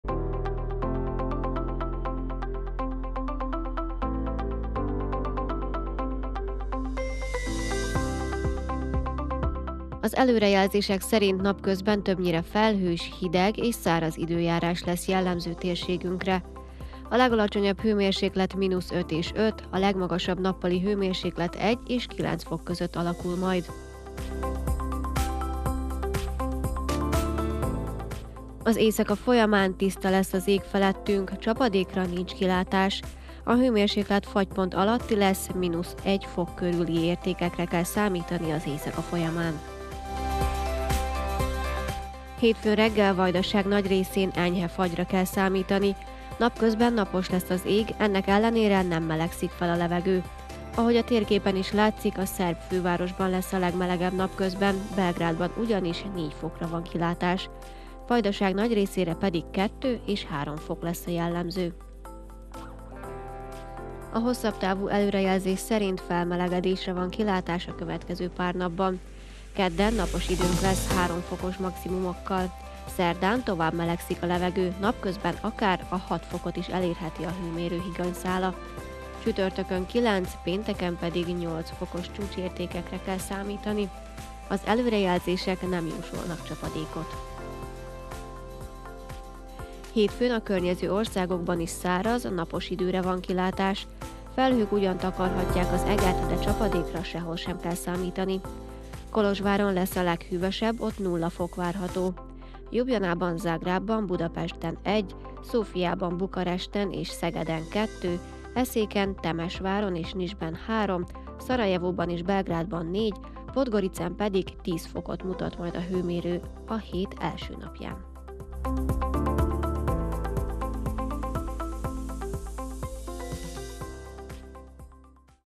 Időjárás – 2022.12.18.
Az előrejelzések szerint napközben többnyire felhős, hideg és száraz időjárás lesz jellemző térségünkre.